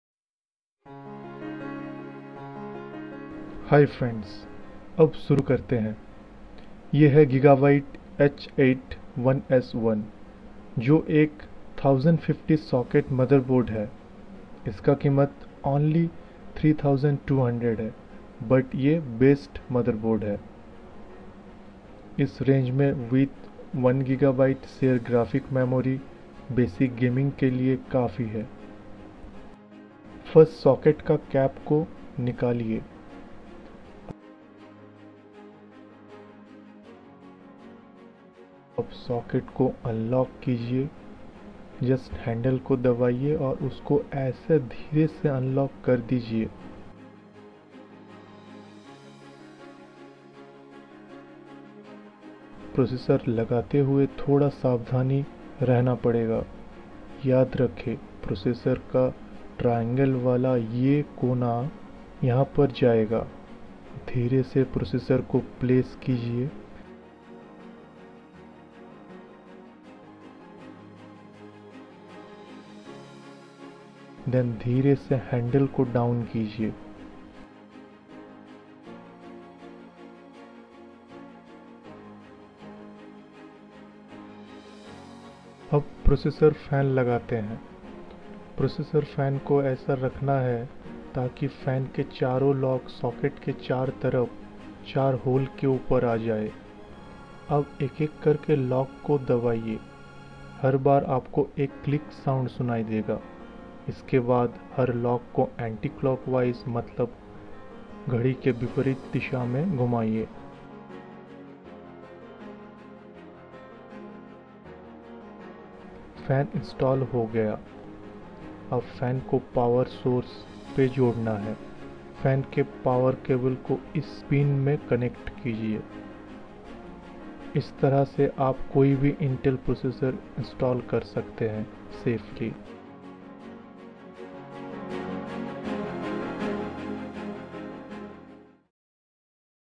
Beginners PC assembling Guide (Hindi): How to install latest Intel Processor safely
This video shows how to install latest Intel processor safely. This video is for amateurs to guide them in safe installation of processor and safe installation of processor fan